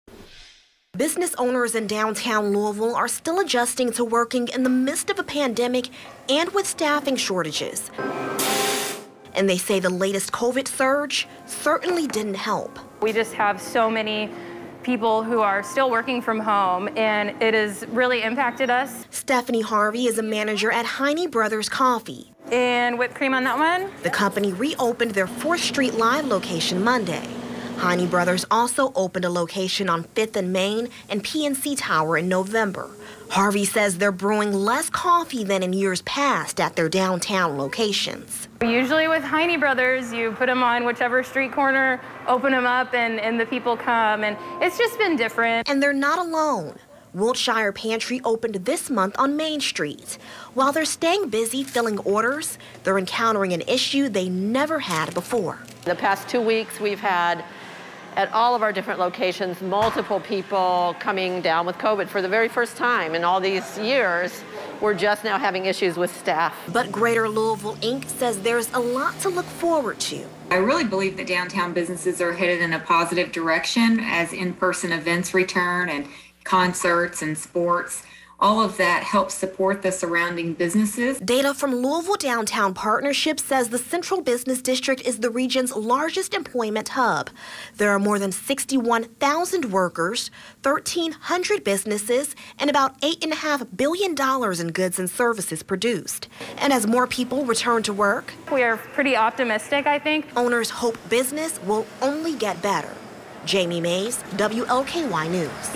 As downtown businesses deal with omicron surge, officials are saying they are moving in the right...
As downtown businesses deal with omicron surge, officials are saying they are moving in the right direction